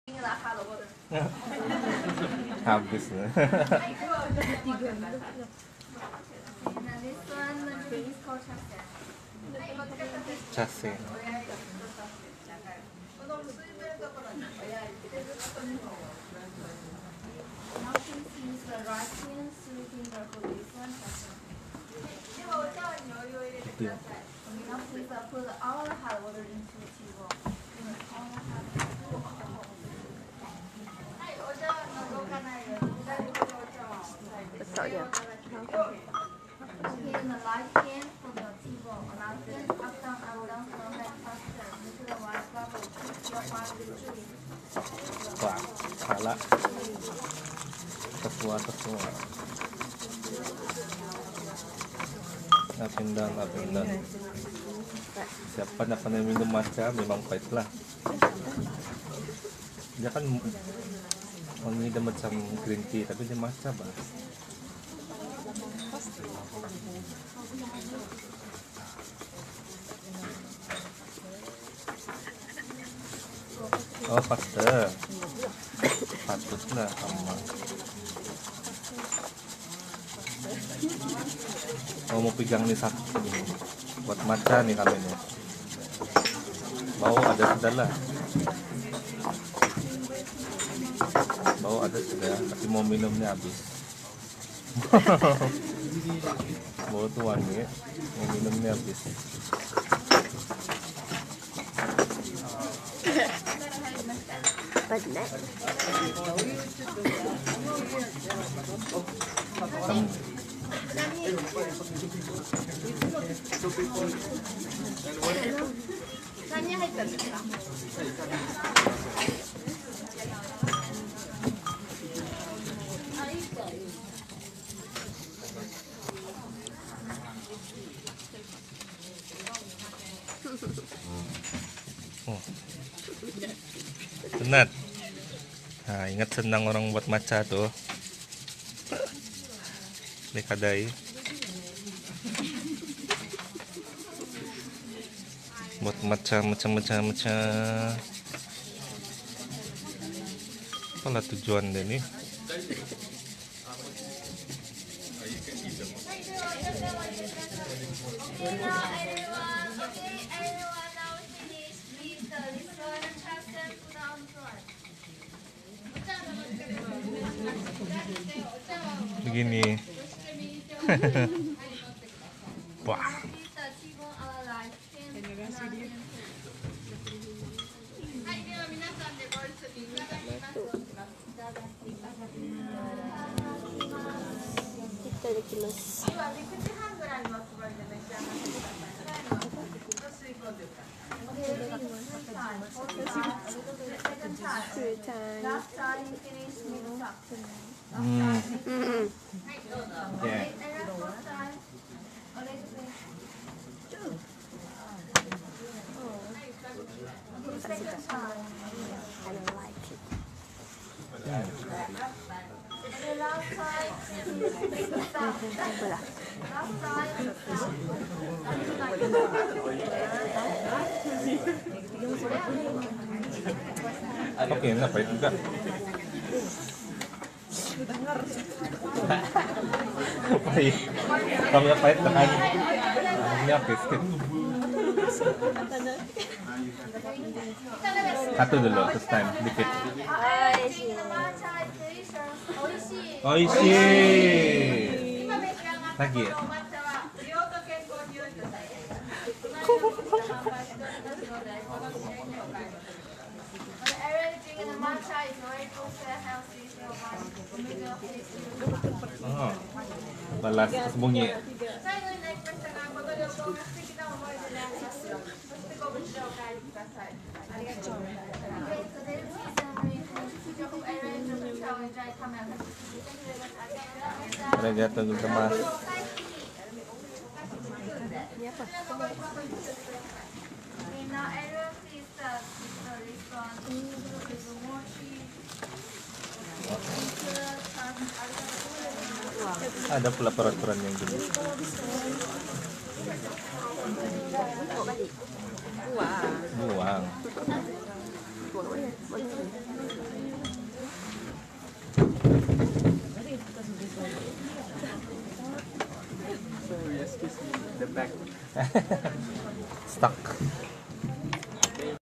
HOW TO MAKE GREEN TEA MATCHA
SIGU SIR TERRY TRAVEL TO OSAKA,JAPAN FOR MATCHA CEREMONY.